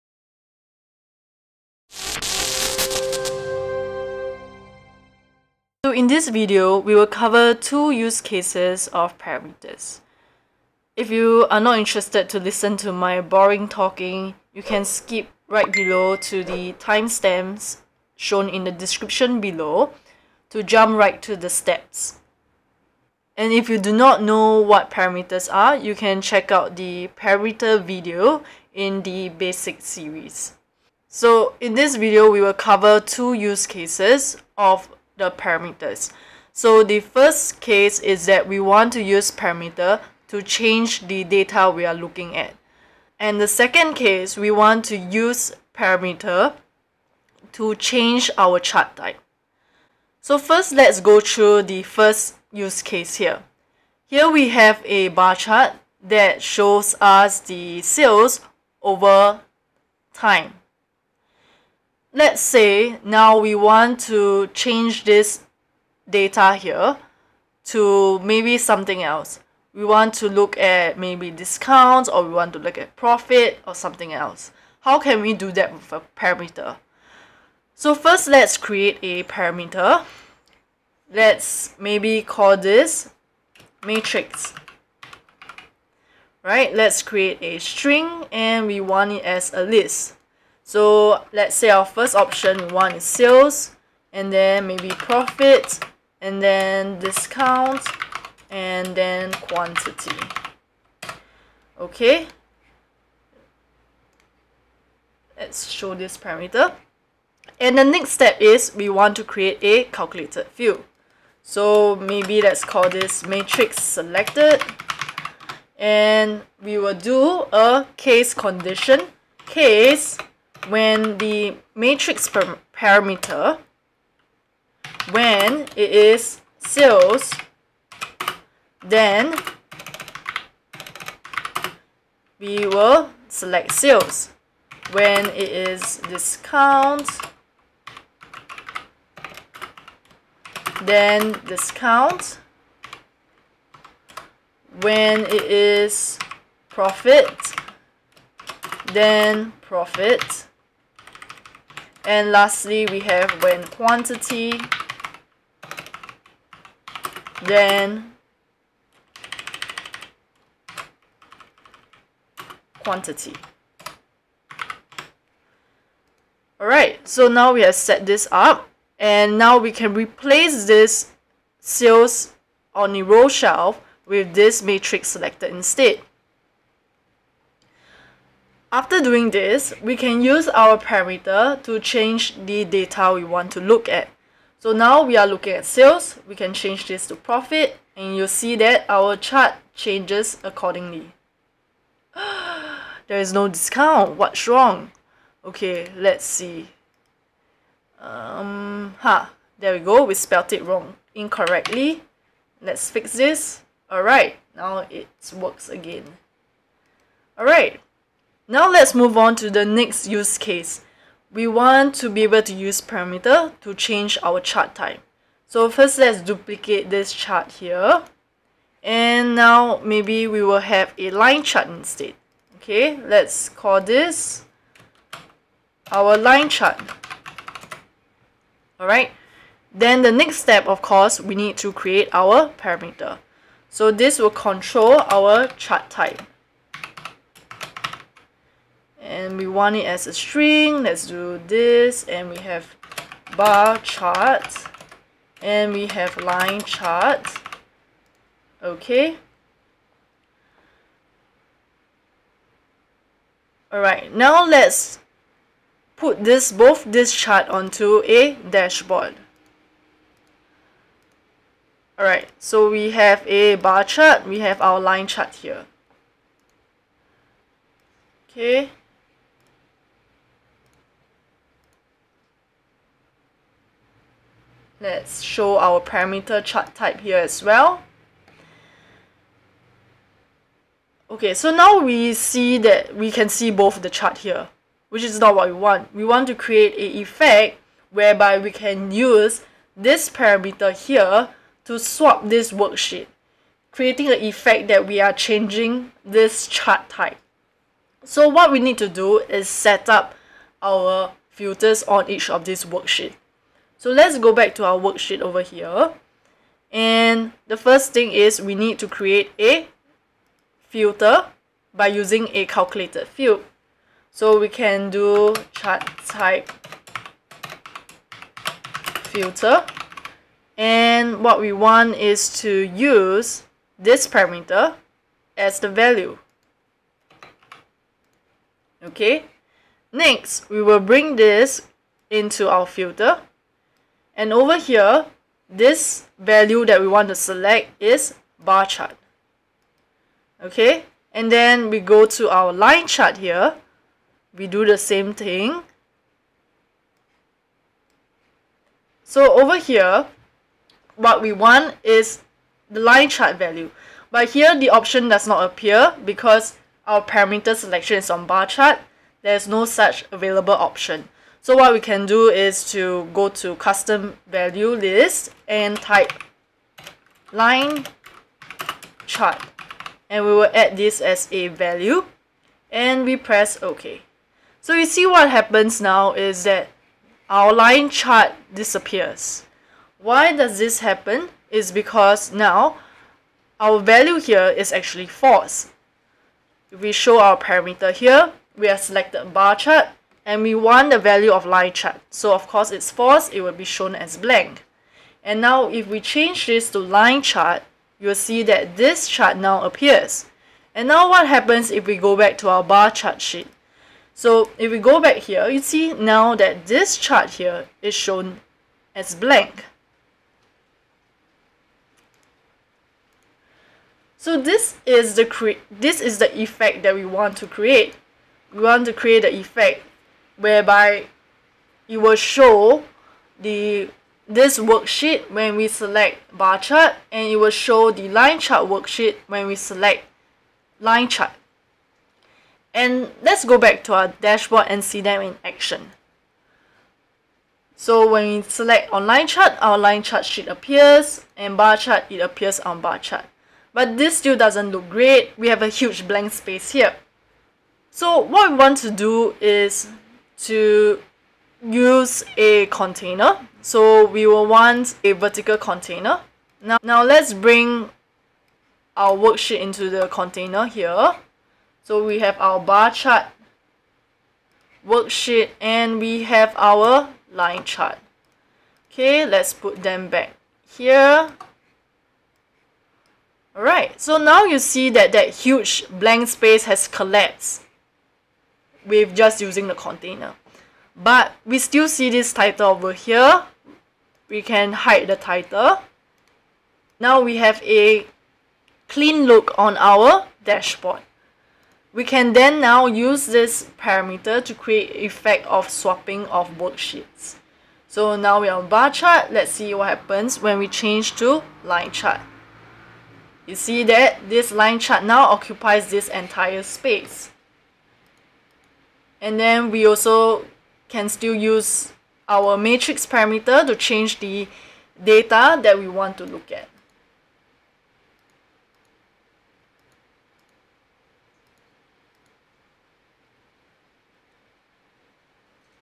Switch/Toggle Charts, Data - Tableau Tips
Here on another episode of tableau tips, learn how to use parameters to swap worksheets to change chart type and data
1:18 Use case 1: change data
3:30 Use case 2: swap sheets , change chart type
7:30 set up dashboard action